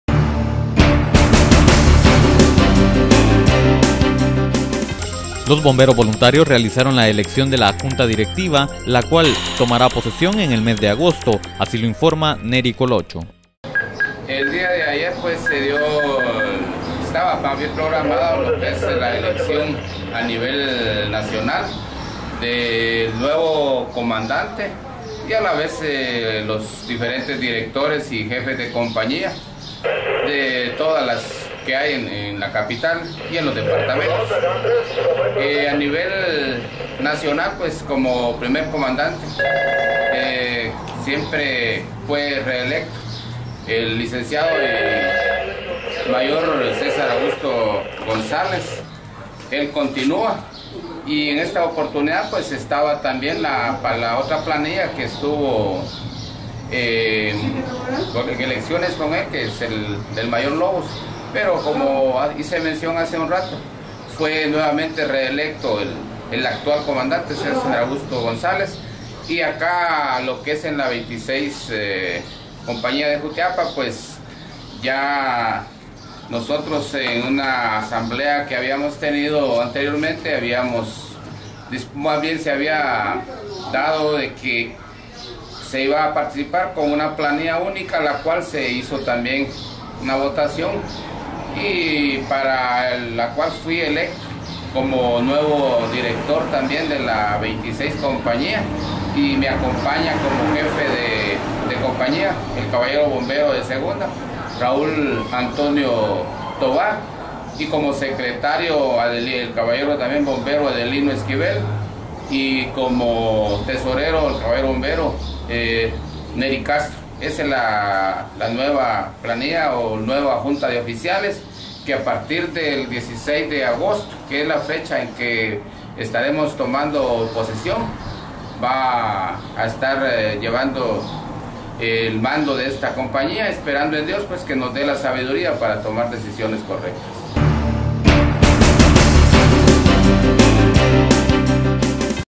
Nueva Directiva en Bomberos de Jutiapa
El día de ayer se realizó la Elección de la Nueva Directiva de los Bomberos Voluntarios
de Jutiapa y el señor Nery Colocho nos da a conocer como quedó conformada esta nueva
organización.